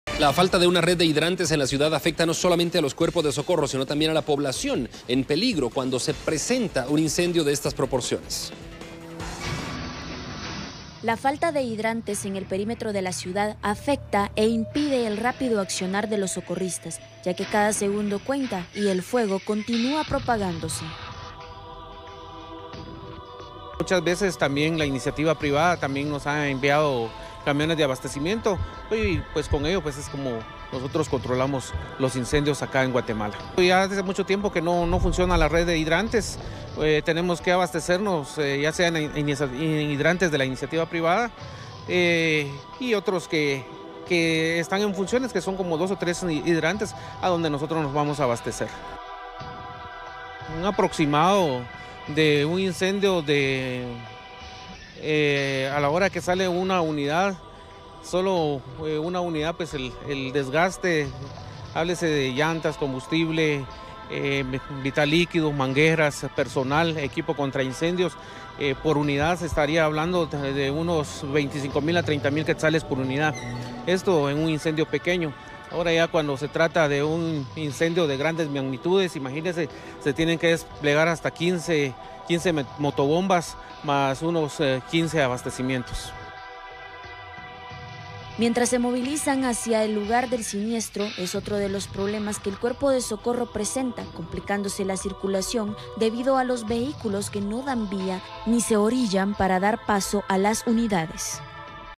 Falta red de hidrantes dificulta combate de incendios
La población es más vulnerable debido a esta carencia